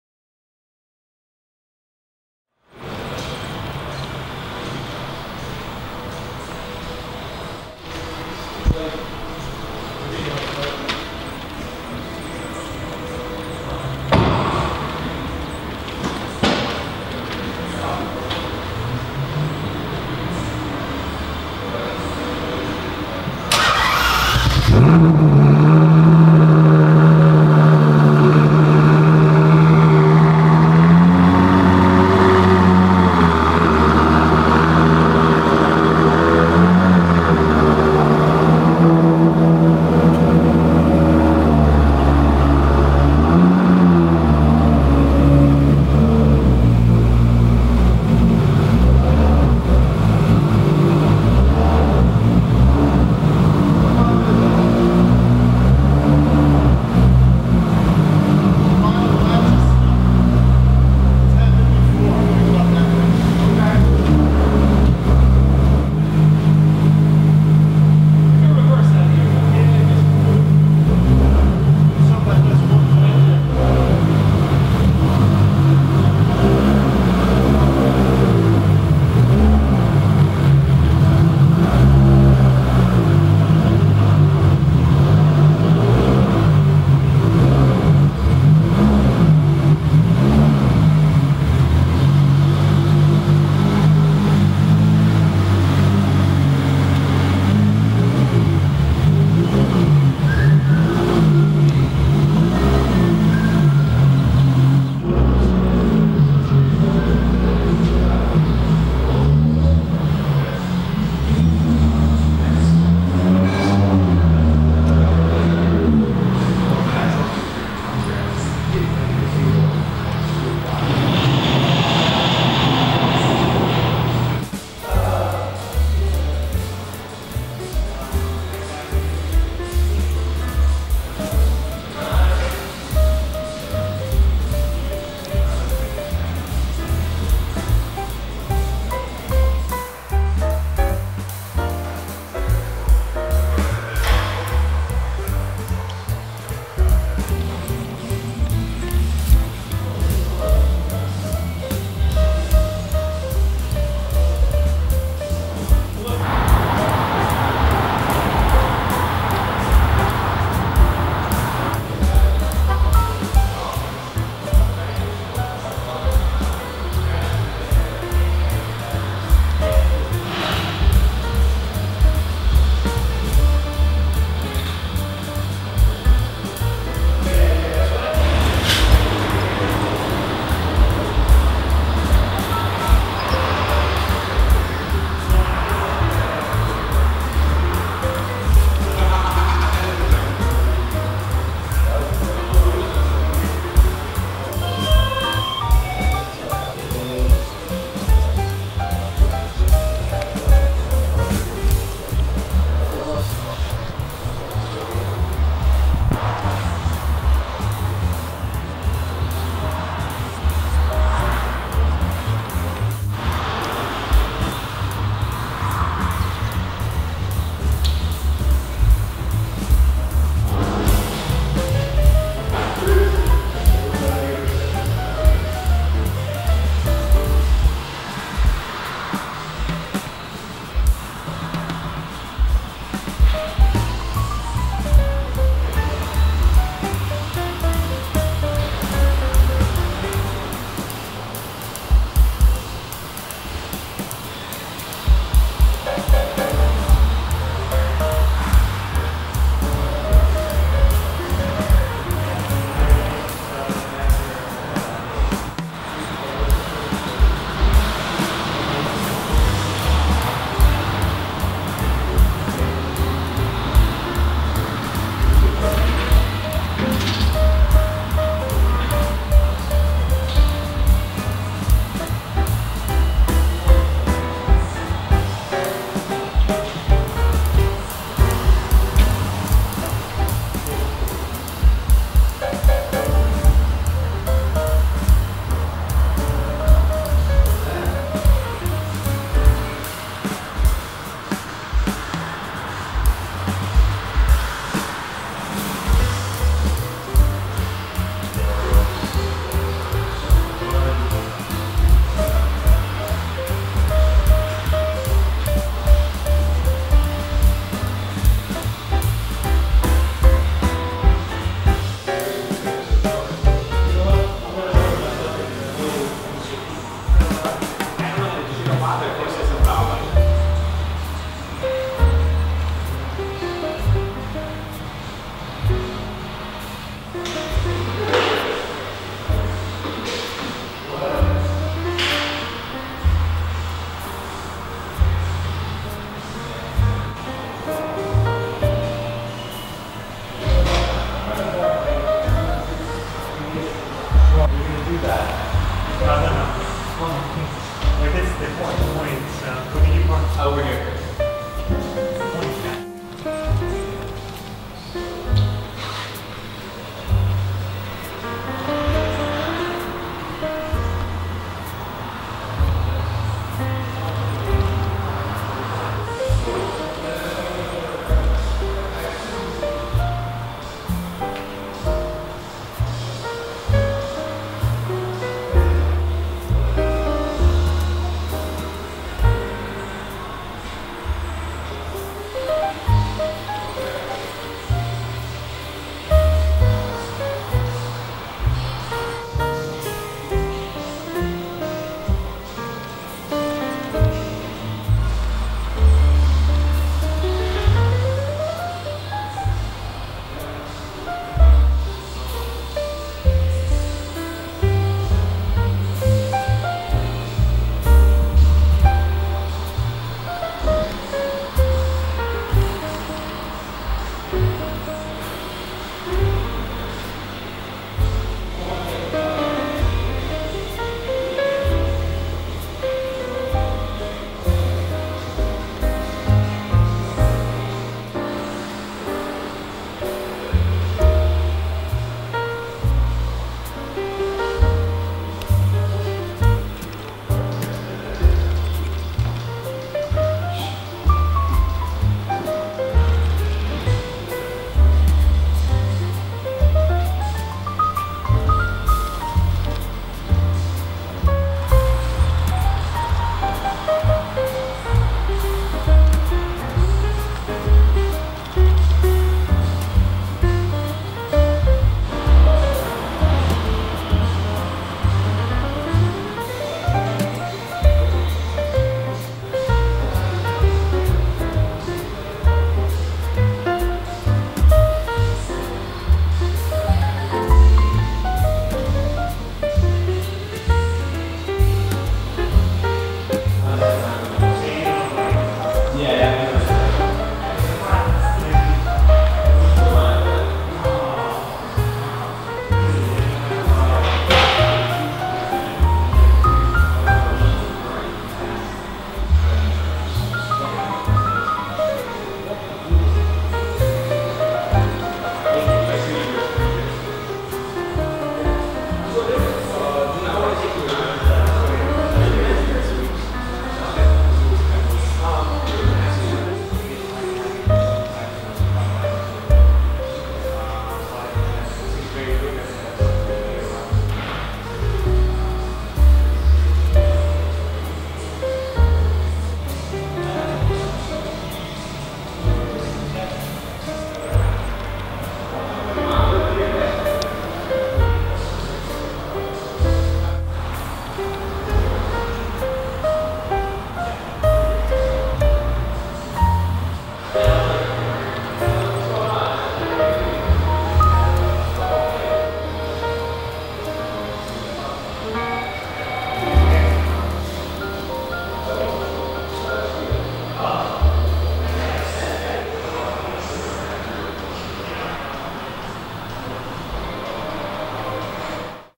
Special Ferraris on Display at Wide World Ferrari
Here are some special Ferraris on display at Wide World Ferrari, including a Monza SP2, Blu Pozzi Enzo, F40, LaFerrari, SP America, F12tdf, and SF90. Enjoy!